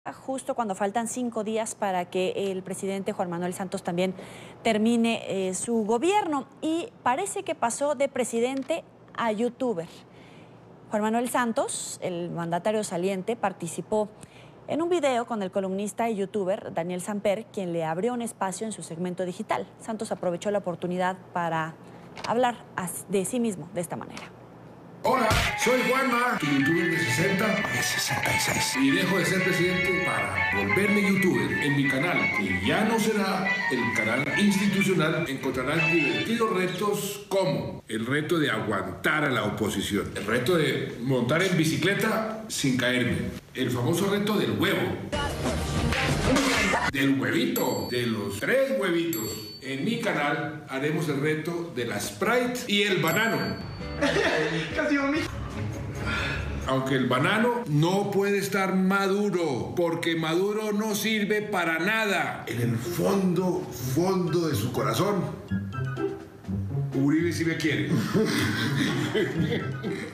Juan Manuel Santos: ¿De presidente a ‘youtuber’?
¿De presidente a ‘youtuber’? Cuando faltan cinco días para que Juan Manuel Santos deje el poder, el saliente mandatario de Colombia participó en un video con el columnista y ‘youtuber’ Daniel Samper, quien le abrió un espacio en su segmento digital.